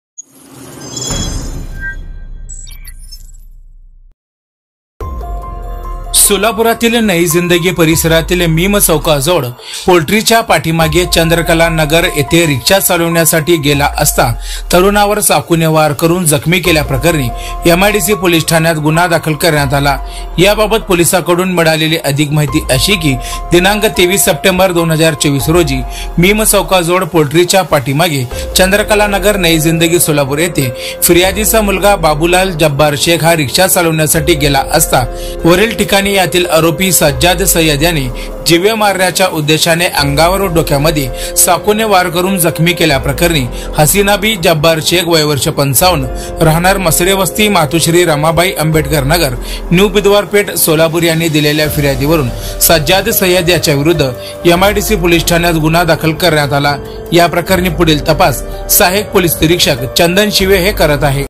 सोलापूर : नई जिंदगी परिसरात तरुणावर चाकूने वार..
Lokshakti News